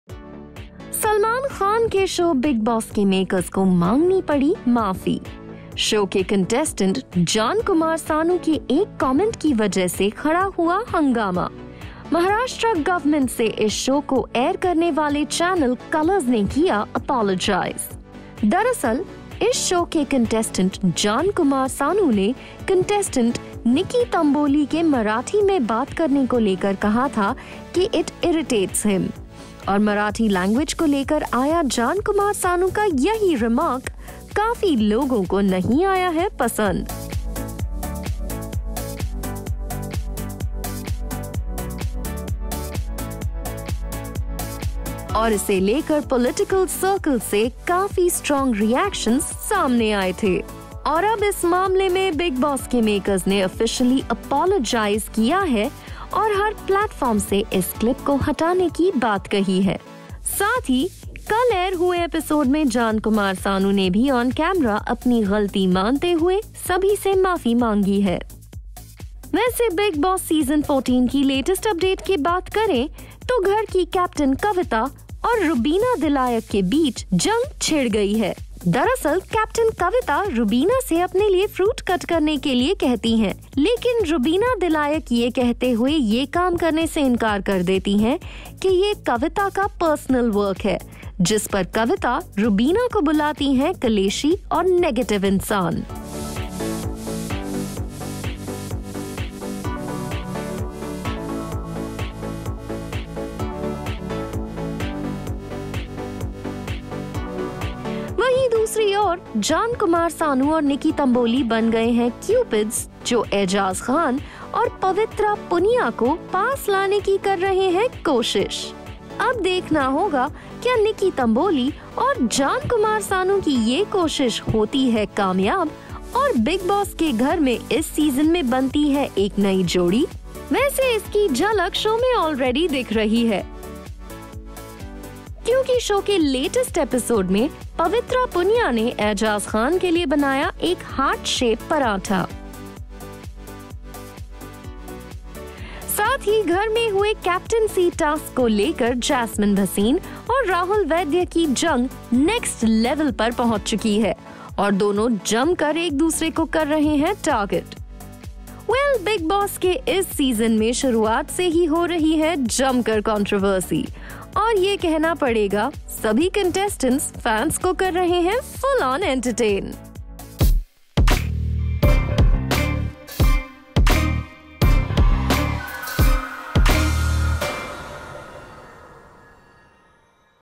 Bigg Boss 14 makers apologize after Jaan Kumar Sanu's remark on Marathi language sparks controversy
The makers of Bigg Boss 14 have extended an apology to Maharashtra Government after contestant Jaan Kumar Sanu's comment on the Marathi language sparked controversy. The comments came to light after Jaan entered into an argument with another contestant Nikki Tamboli and asked her to restrict herself from speaking in Marathi in front of him. He even told her that he gets irritated on hearing the language and if she has the courage, she should speak in Hindi. The show faced a lot of backlash from the political segments after it was aired on the channel. Listen In!